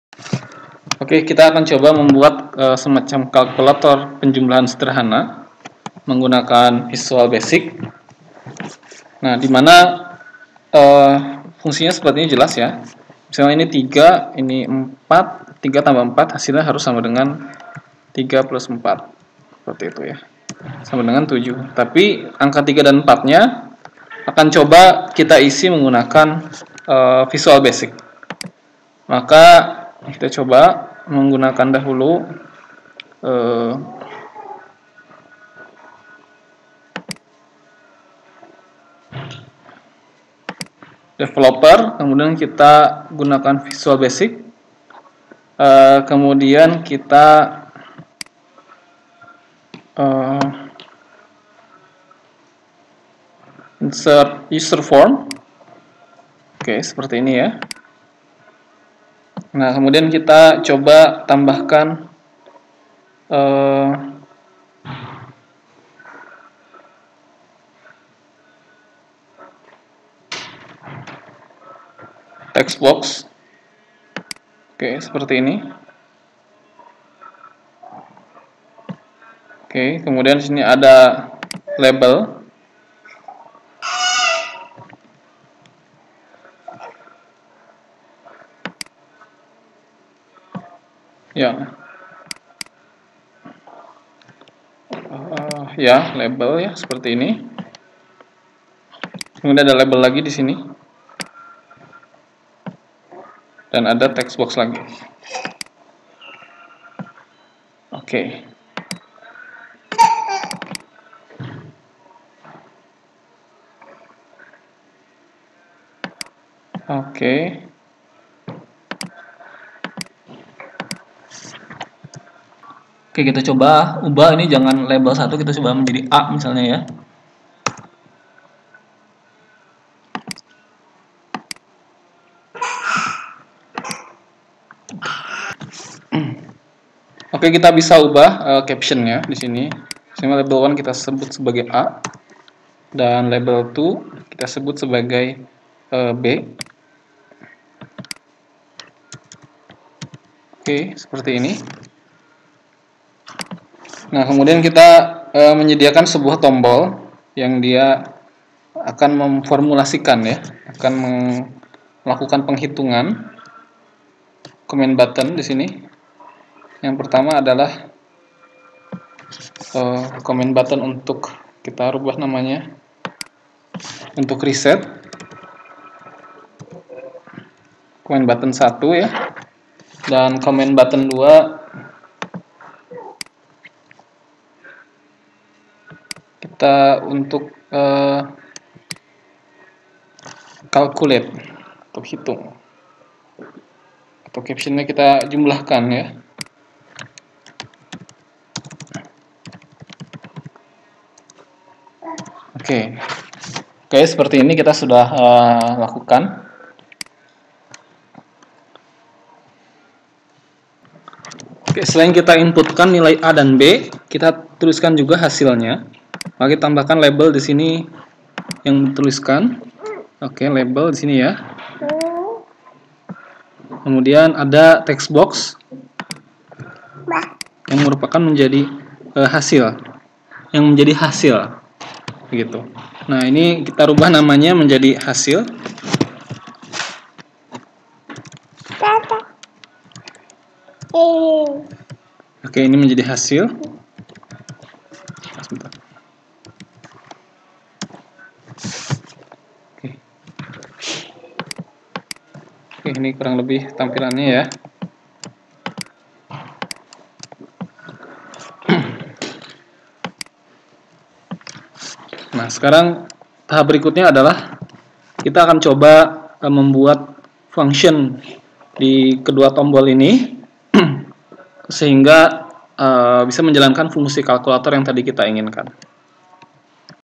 Aplikasi Sederhana dengan Excel VBA DSS 1 membuat layout userform
TAWARAN 1
Bila membutuhkan bantuan untuk jasa pembuatan aplikasi menggunakan visual basic dan excel
Dapat menghubungi laboratorium kami: